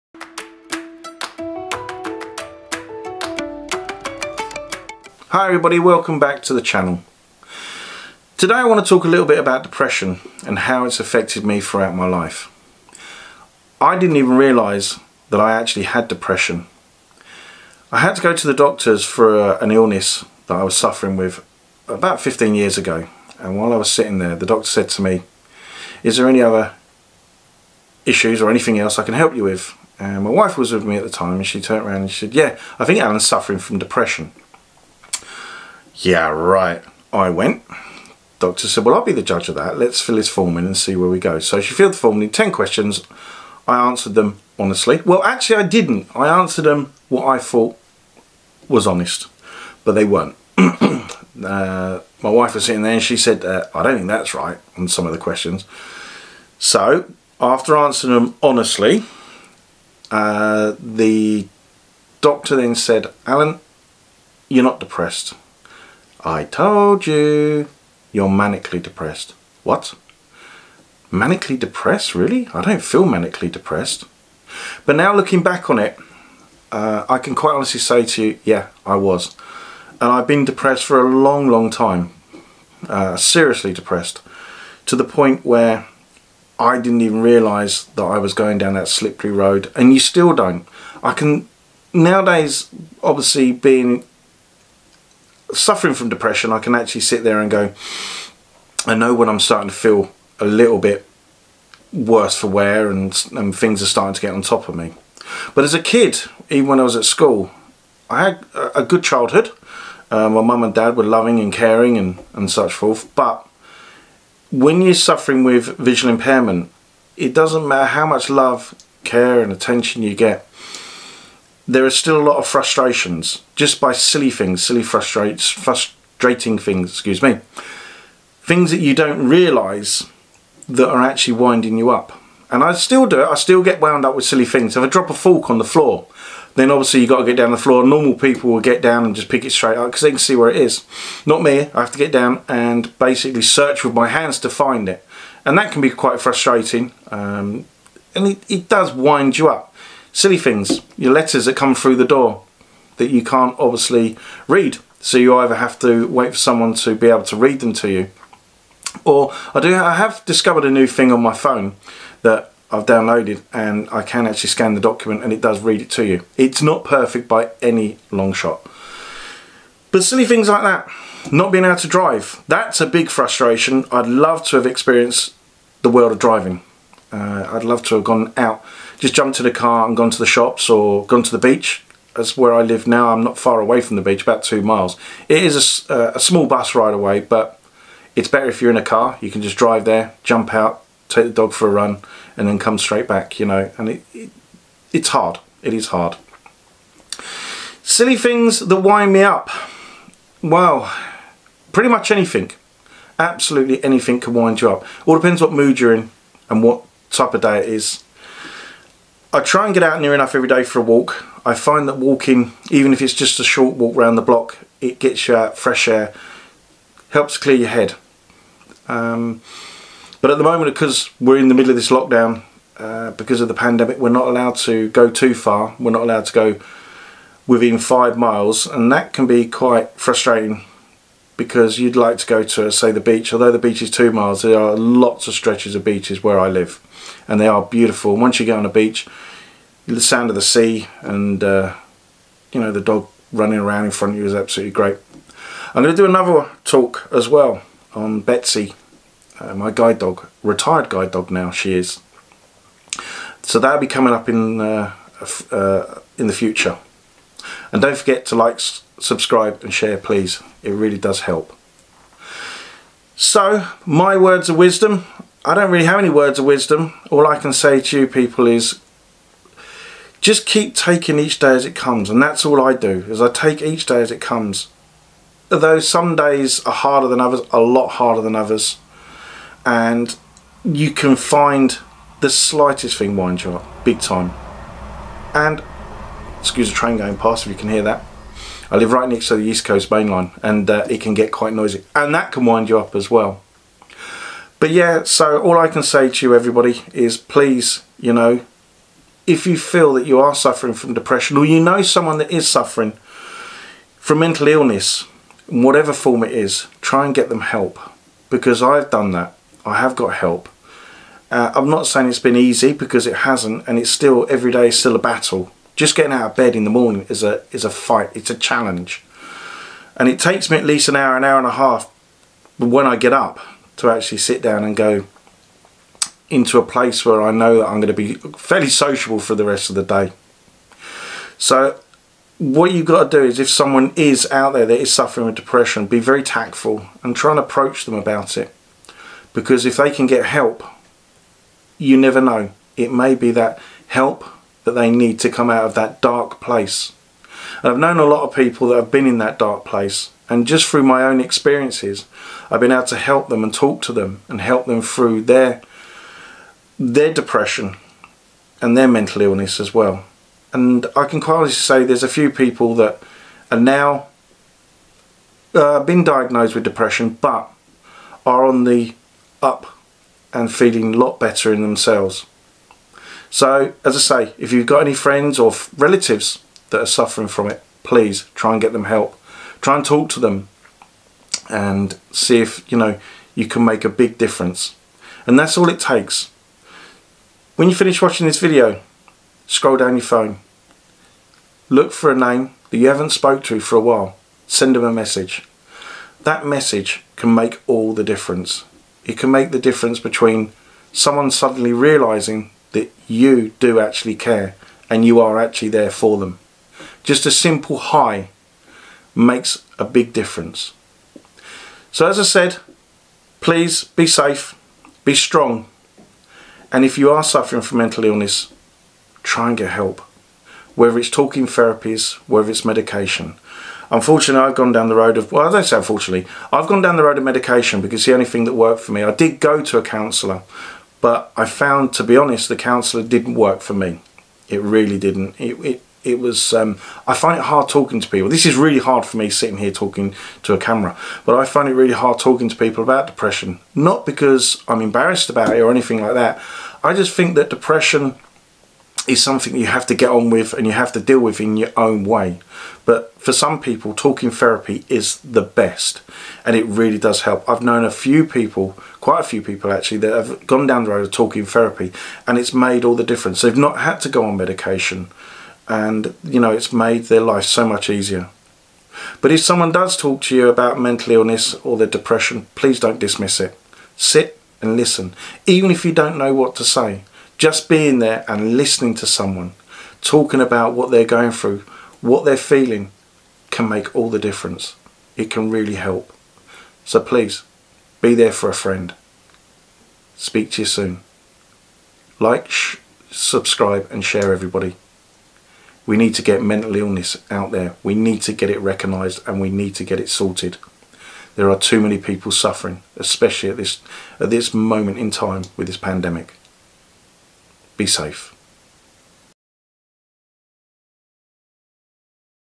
Depression And Me
Hi everyone Welcome back to another Vlog where I talk about my depression and living with a disability. Its not easy to live with a disability and depression together but we stay strong and I hope this can motivate or even help others feel like they are not alone.